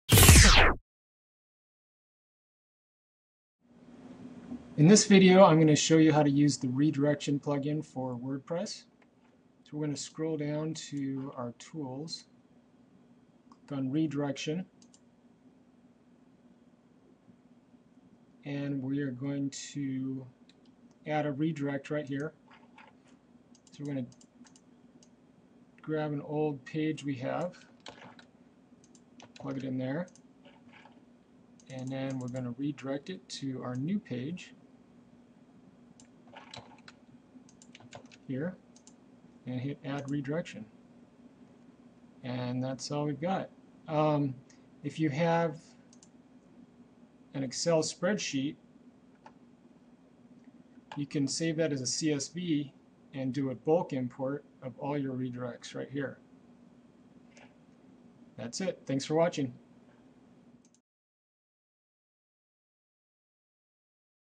WordPress Redirection Plugin - Easy 301 Redirects
- How to use the Redirection plugin for Wordpress